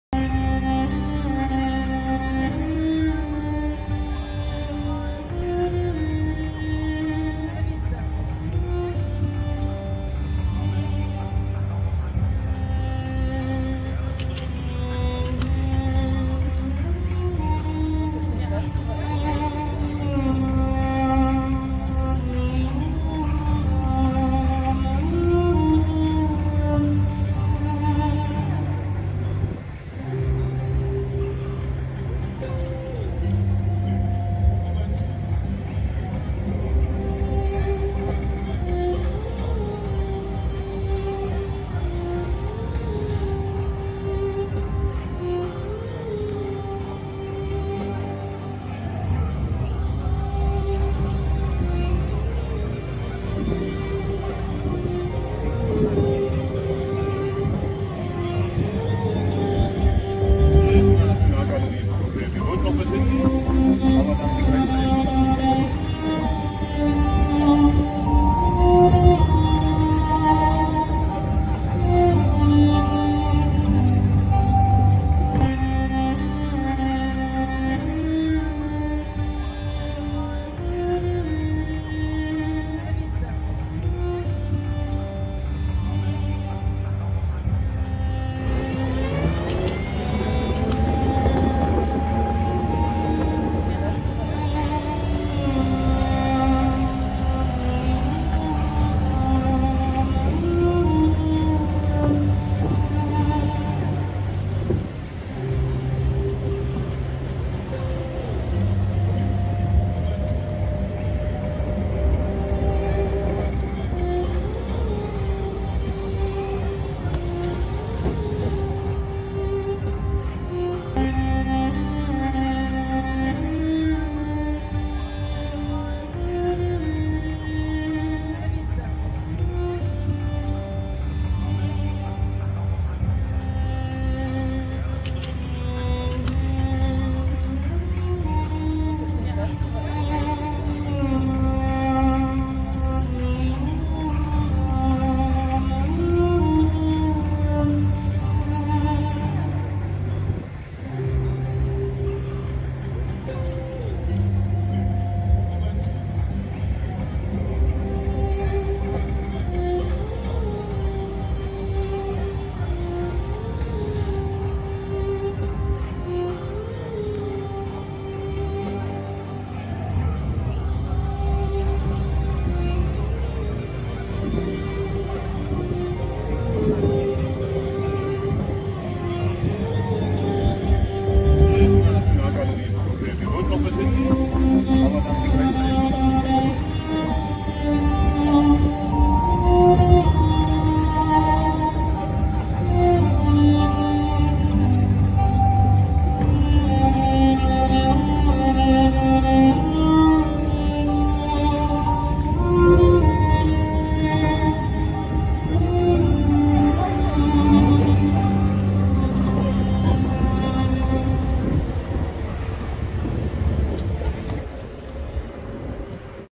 Roller Tour mit TIER durch das abendliche Berlin
hier eine kleine kurze abendliche Tour am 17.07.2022 mit dem TIER Elektroroller durch das BrandenburgerTor und Unter den Linden entlang zum Checkpoint Charlie.